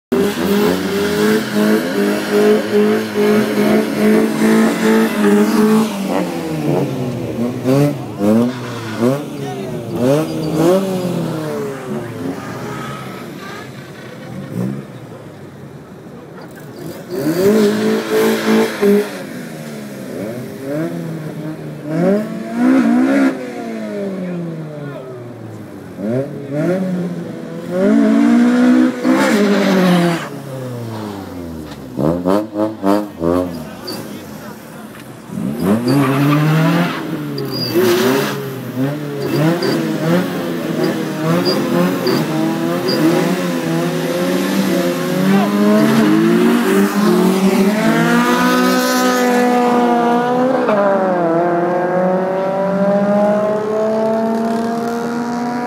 Philly streetracing Ek vs Ef
Ek coupe(owner) vs ef ...ef wins